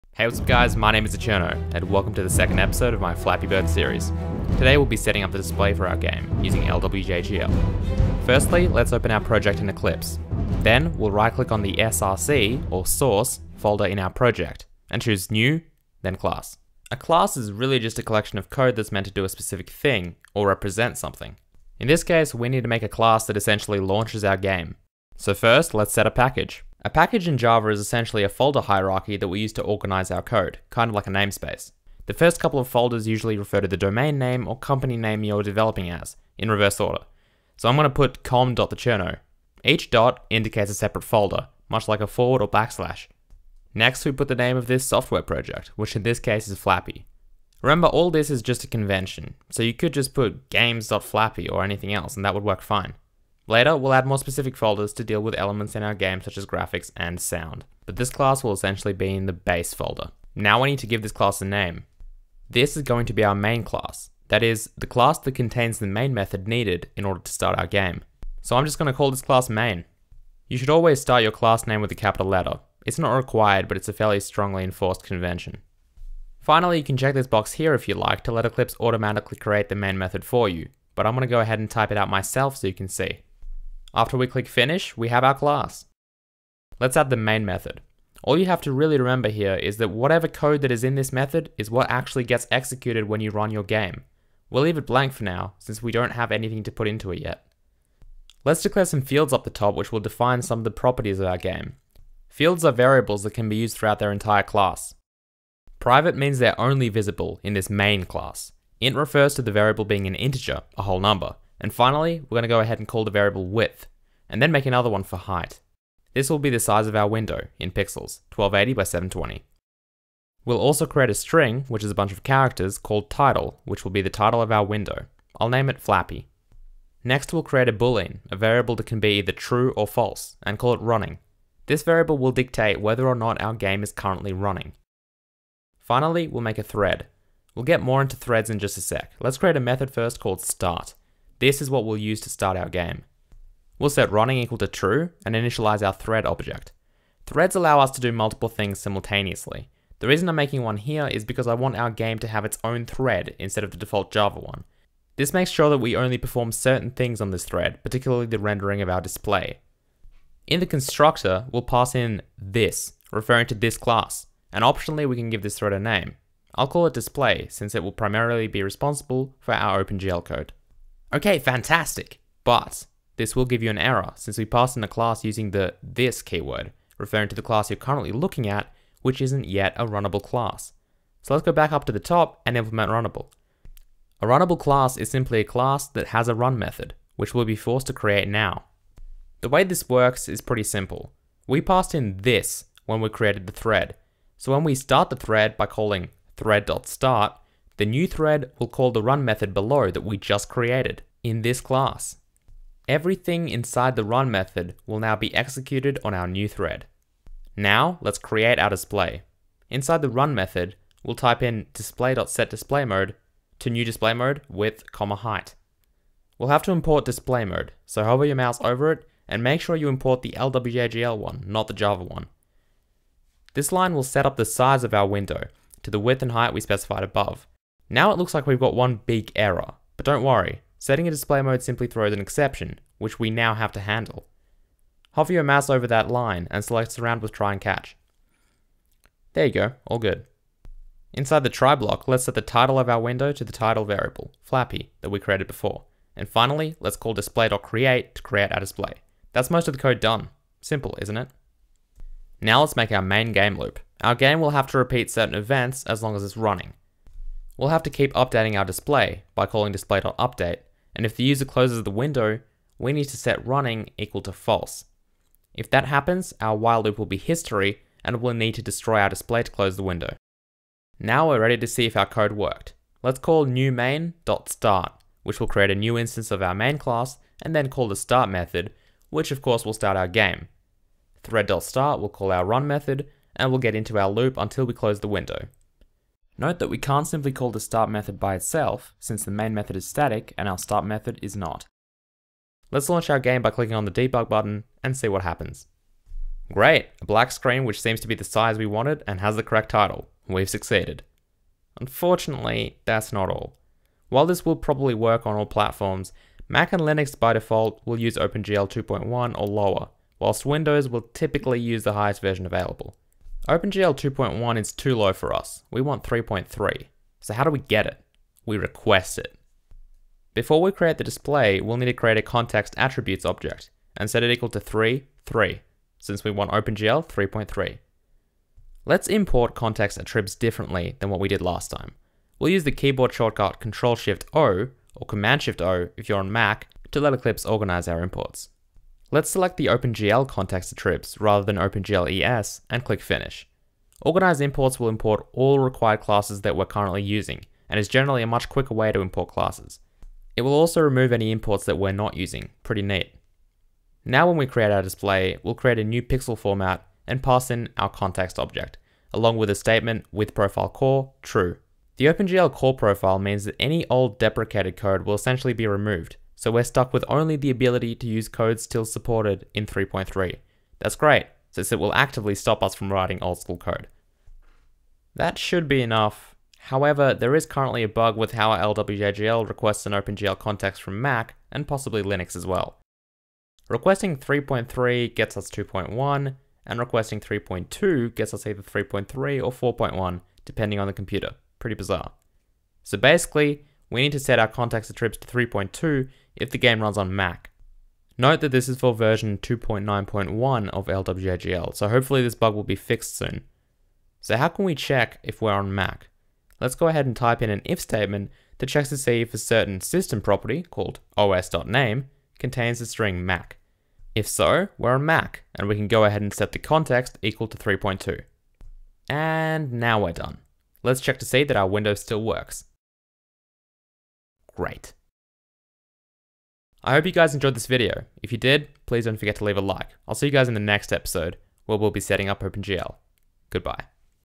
Ep. 2: Display - Let's Make Flappy Bird
Let's Make Flappy Bird is a series on how to make a game like the popular mobile game "Flappy Bird", from scratch.

We'll be making it for Windows, Mac and Linux, using modern OpenGL, using Java and LWJGL. This series isn't designed to teach you Java or OpenGL, but rather teach you how to USE those two technologies to make a rather simple game, using modern techniques.